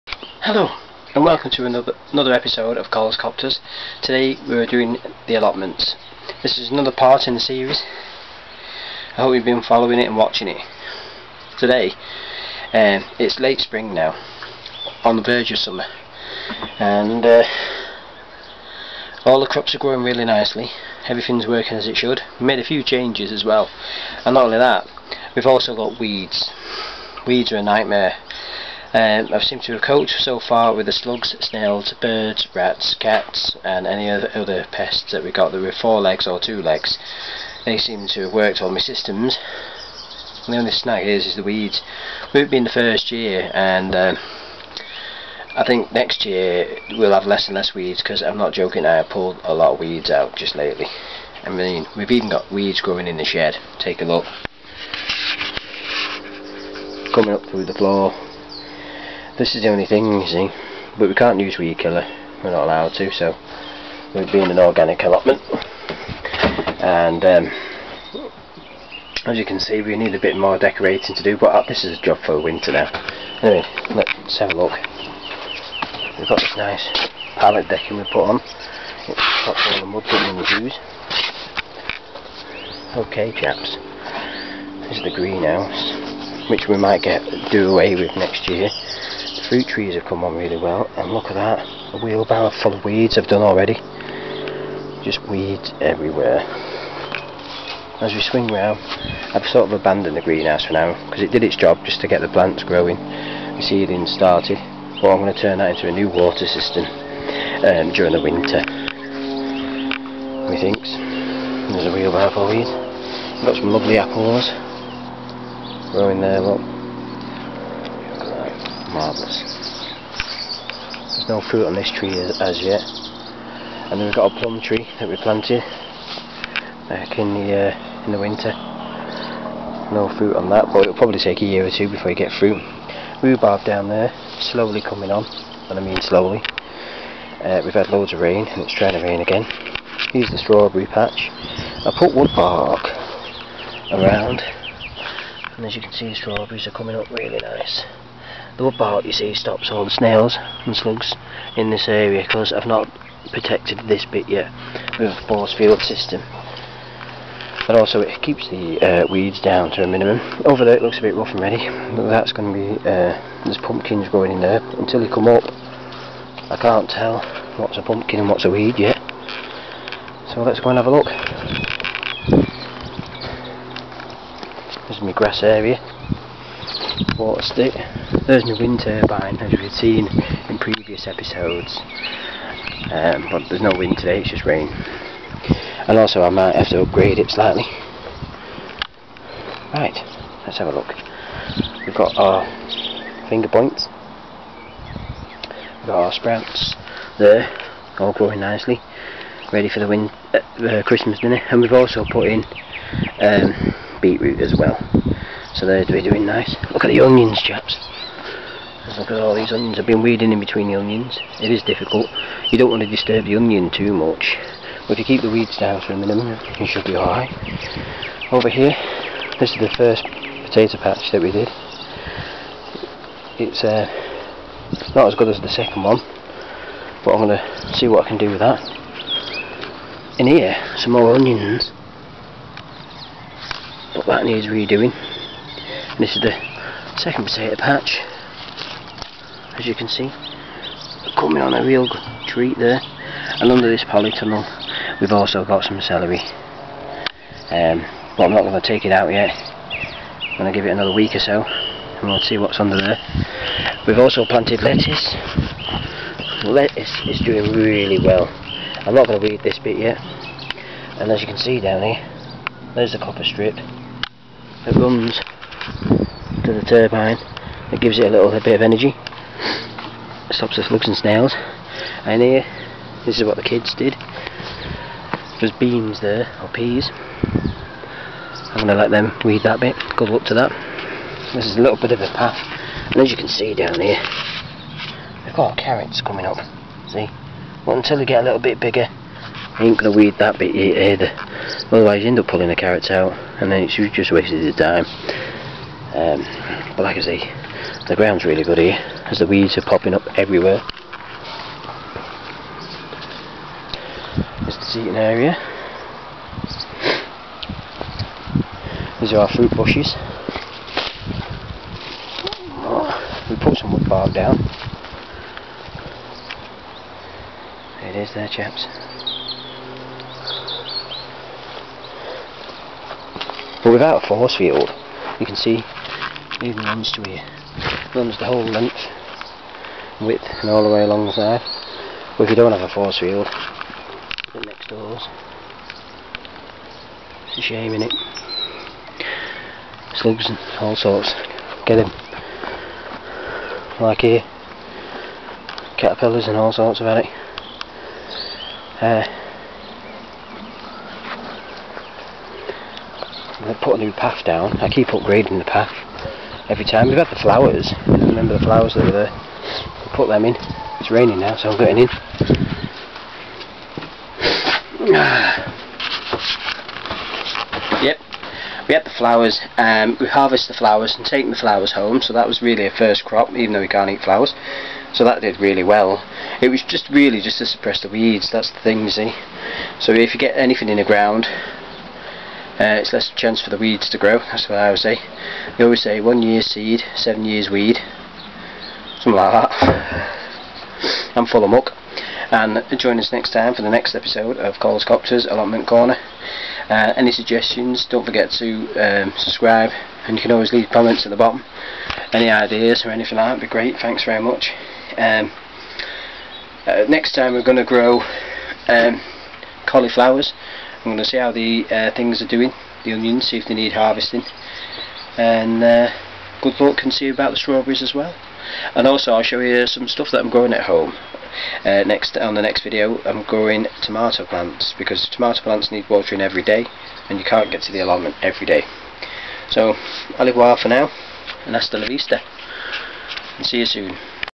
colscopters allotment corner part 5 attack of the weeds !
well folks its late spring look how its all changed but the weeds are fighting back